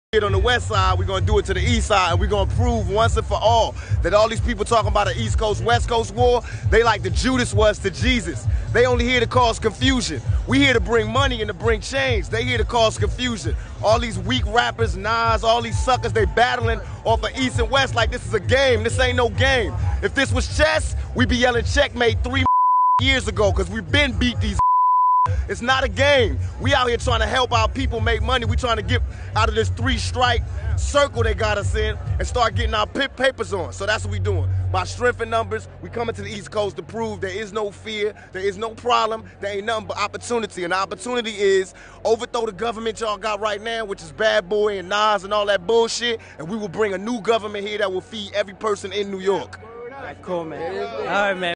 Tupac on Death Row East
Tupac speaking on Death Row East & Death row West @DeathRowChannel @snoopdogg @CRIPWALKX @Tupacking @2PacOfficialYT @ThugTheory@WarrenG